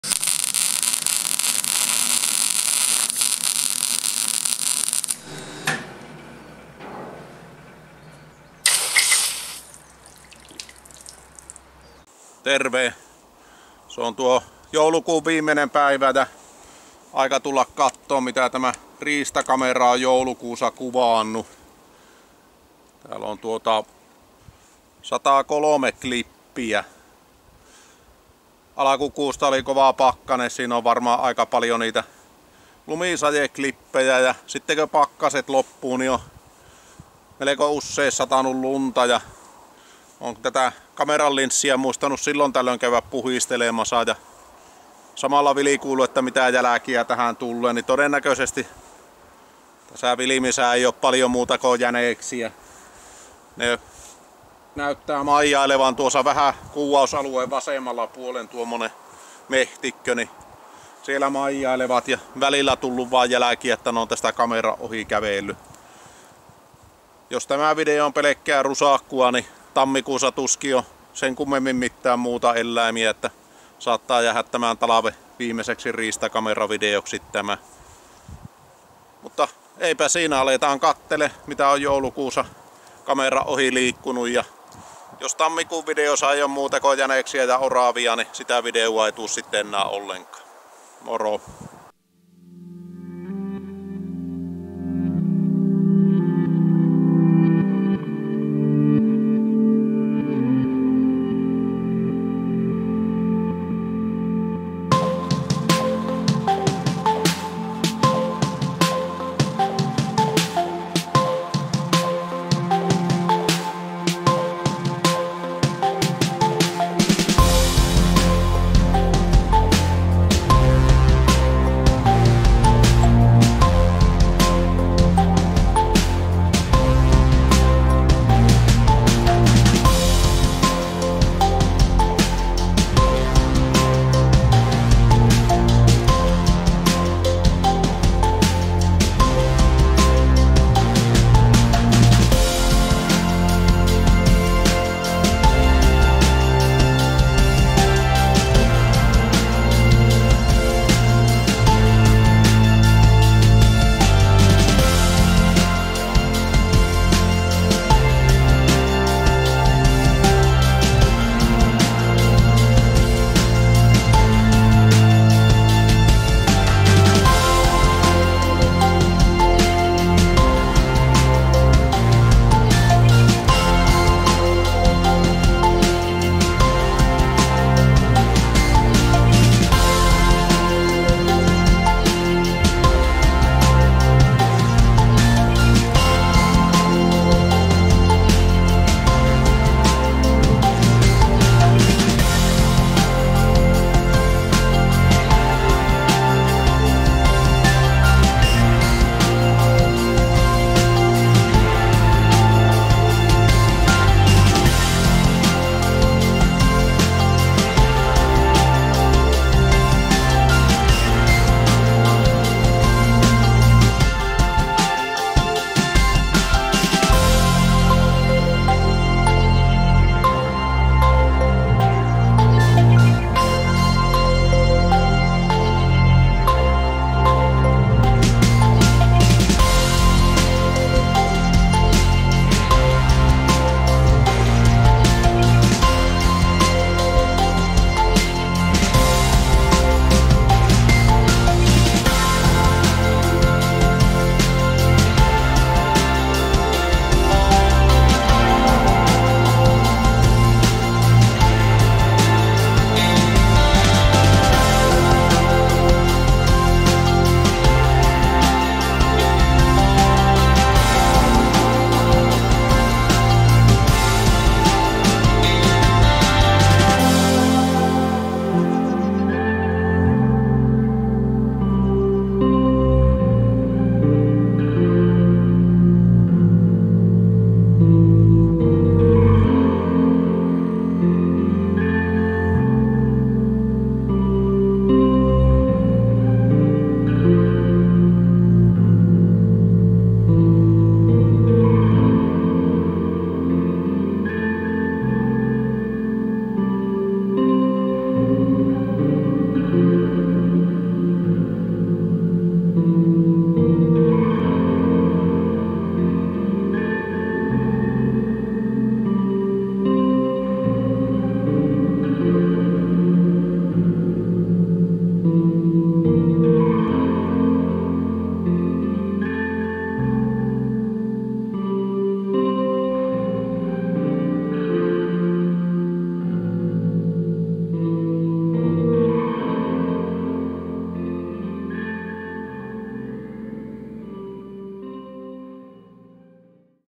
Joulukuun 2021 riistakamerakuvaukset
Sama homma joka vuosi, syksyn kääntyessä talveksi alkaa eläimet vähenemään ja yksipuolistumaan. No, on siellä yksi kettu, mutta muuten pelkkiä jäniksiä.